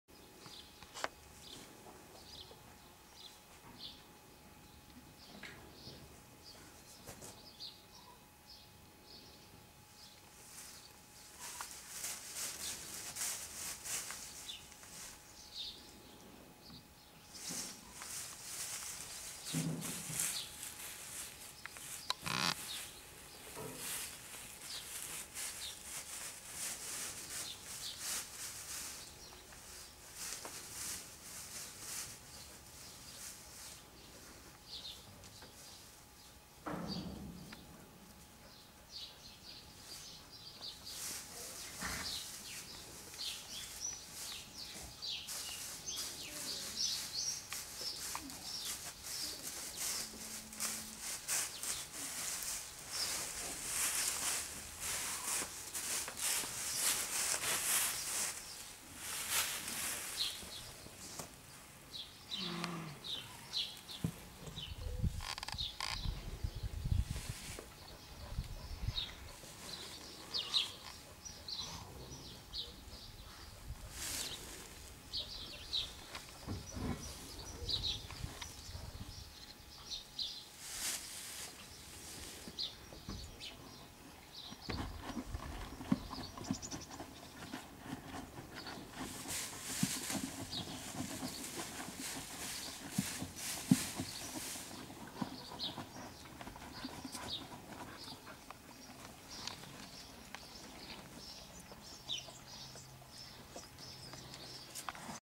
Mare and foal
A young filly born to one of a friends' ex dressage horses. So cute!